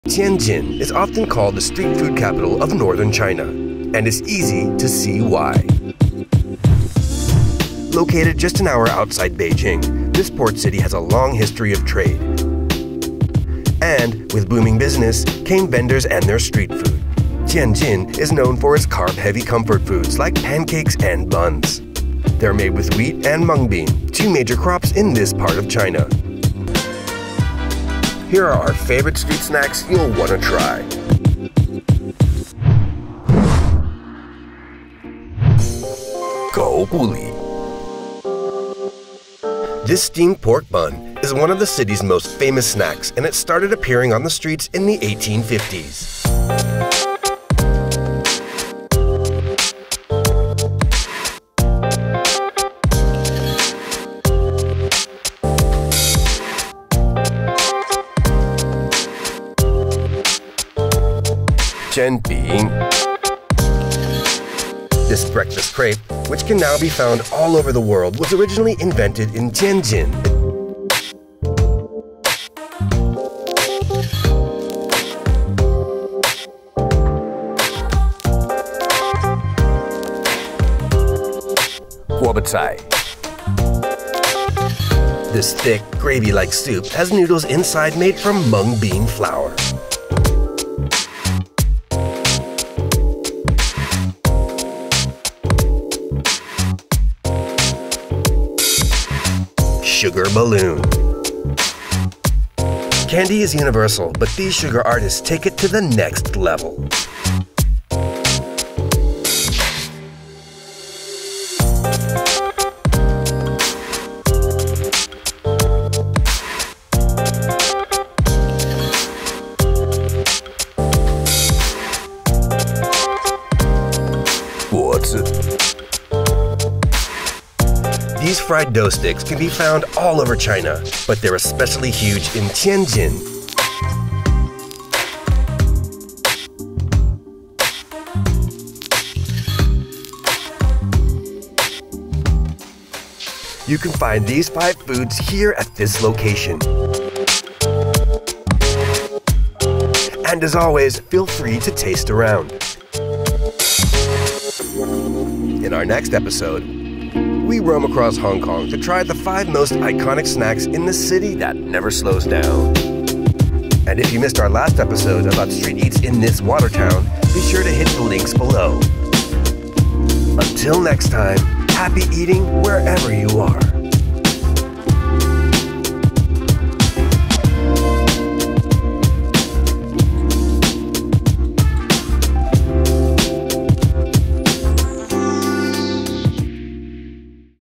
5 Street Food Dishes You Must Try in Tianjin
Tianjin is often considered the street food capital of northern China, and it’s easy to see why.

Located just outside Beijing, this port city has a long history of trade, and with the booming business came street vendors and their food.

The city is known for carb-heavy comfort dishes like mung bean pancakes and pork buns. We checked out five must-visit spots.

If you liked this video, we have more from our 5 Street Eats series in China, including:

Producer and Videographer: Hanley Chu
Writer: Clarissa Wei
Narrator, Editor, and Mastering: Victor Peña